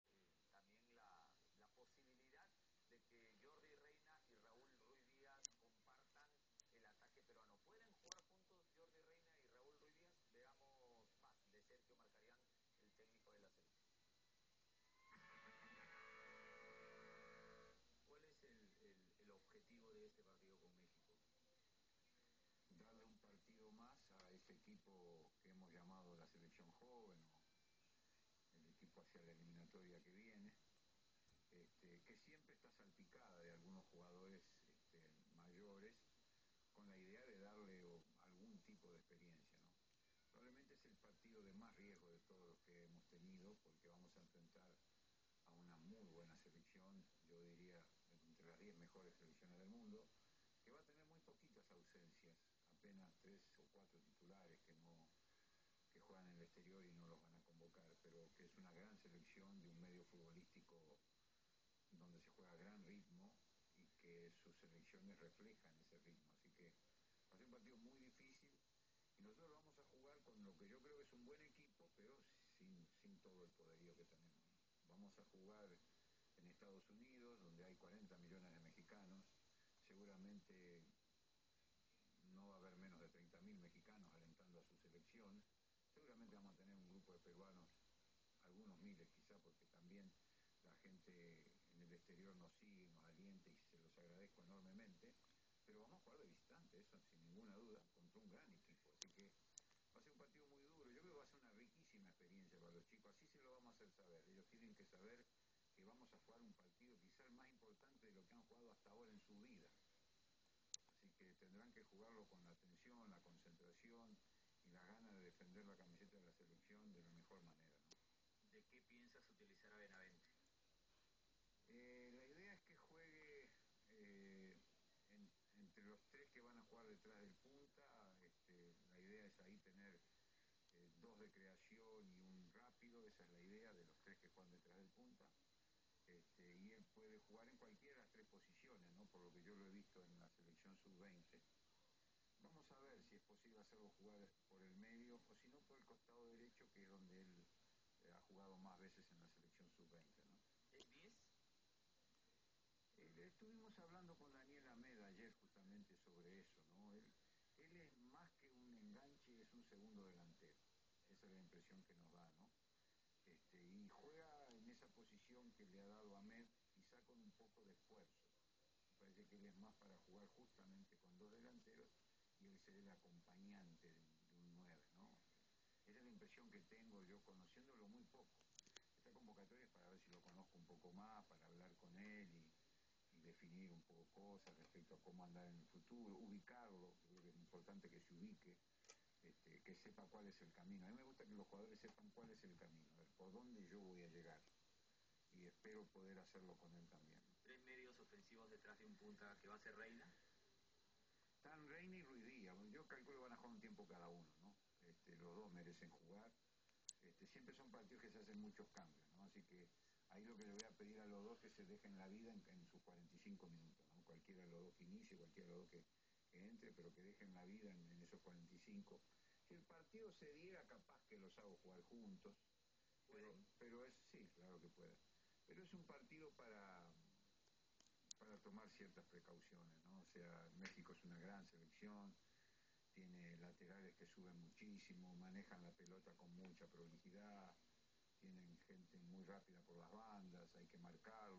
markarian habal sobre cristian benavente
HABLA DE QUE LO PONDRA Y QUE ESTA MUY CONTENTO QUE  JUEGUE POR PERU